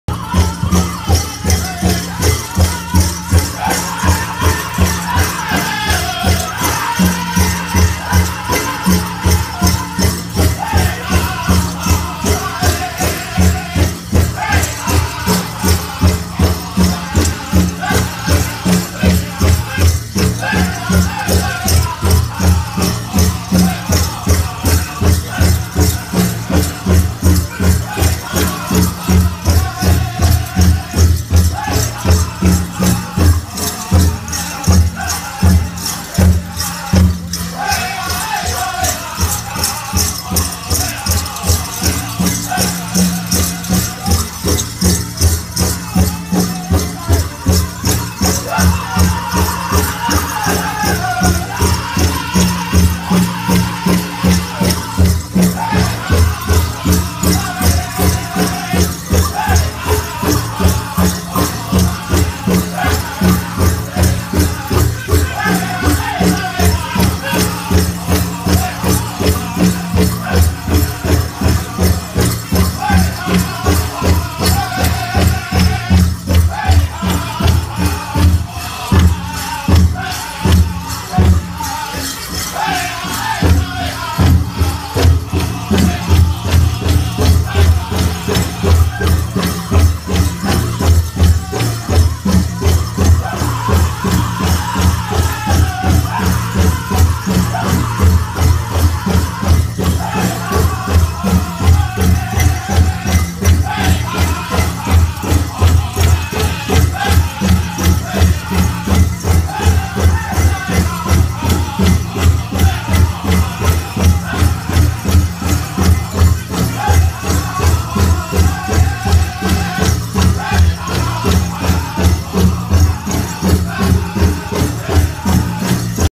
Jr men's grassdance thunderchild 2017 powwow
Me dancing grassdance during the thunderchild 2017 powwow